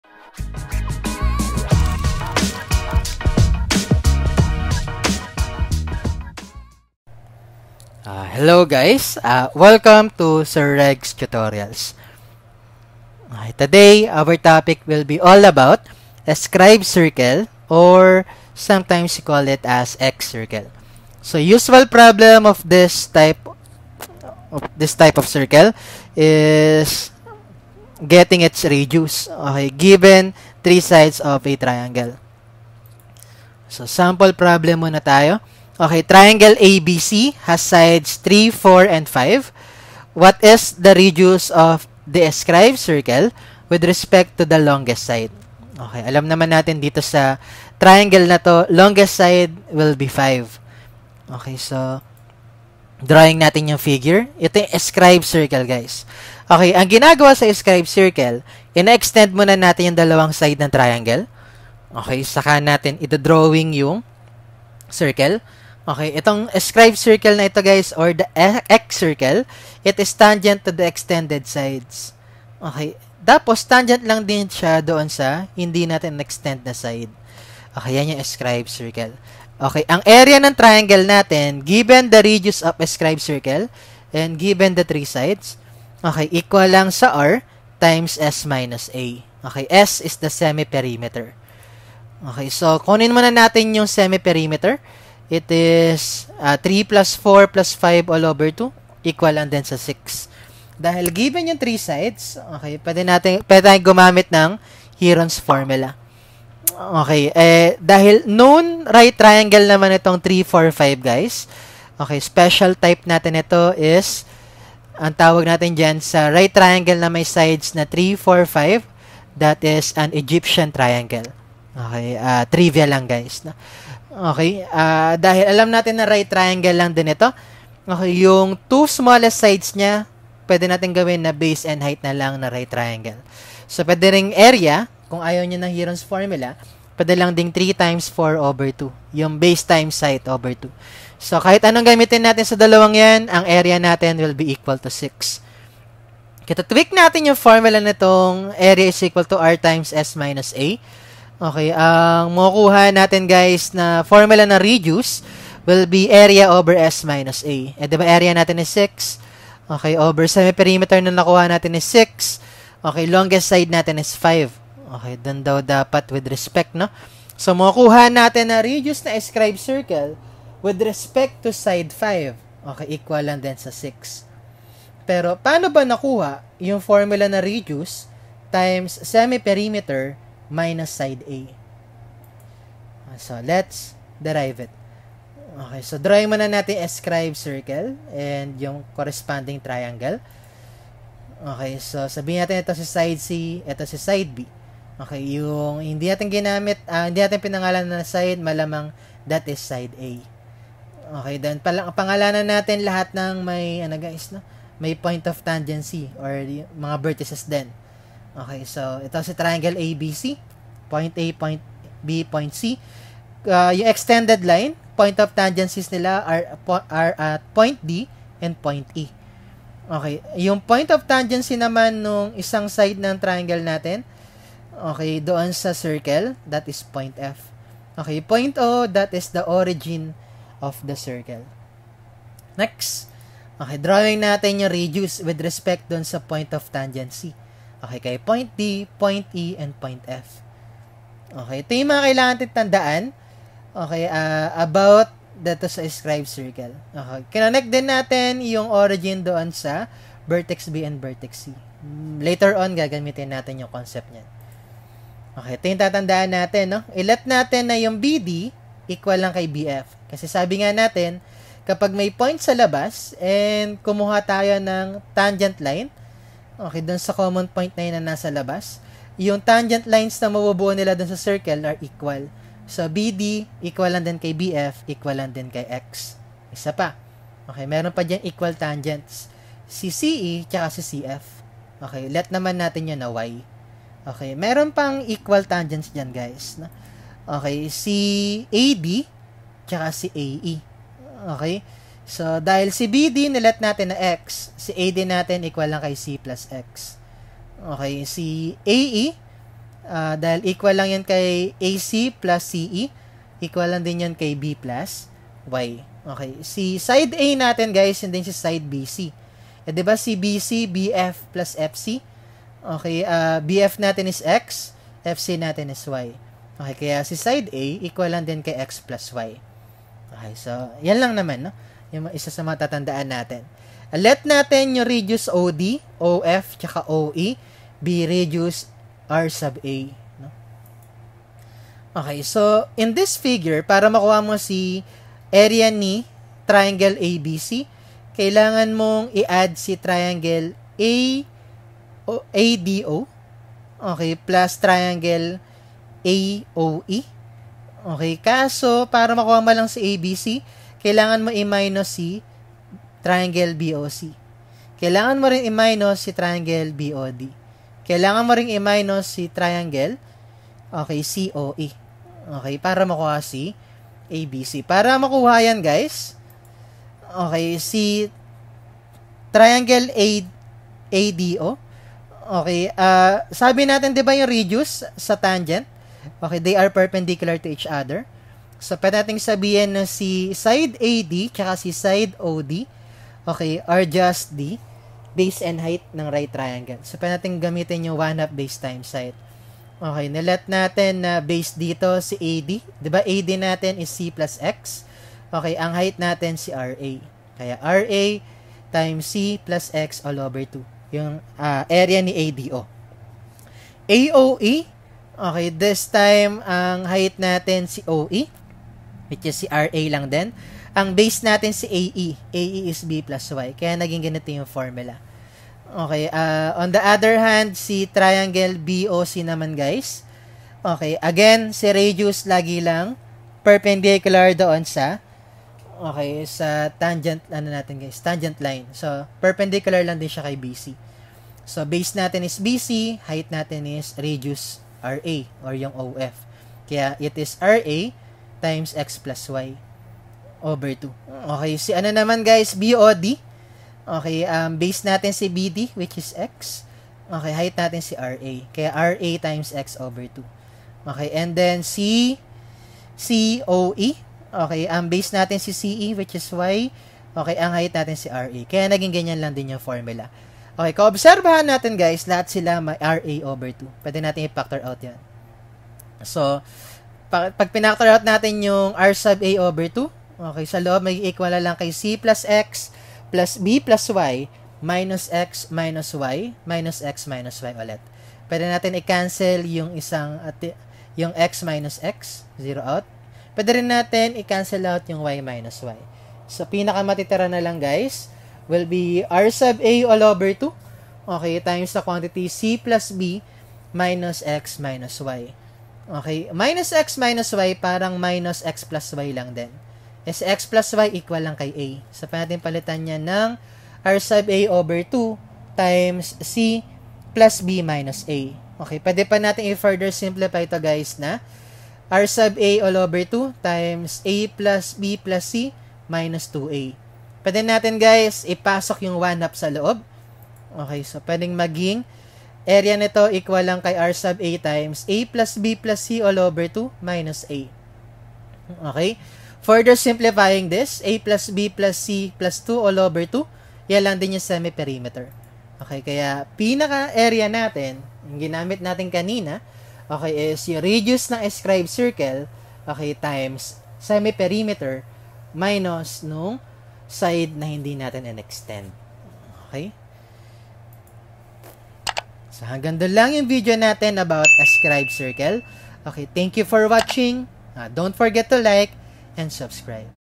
Escribed Circle (or Excircle)
[Tagalog/English] Learn how to use and derive the formula of area of triangle using radius of escribed circle and three sides of triangle.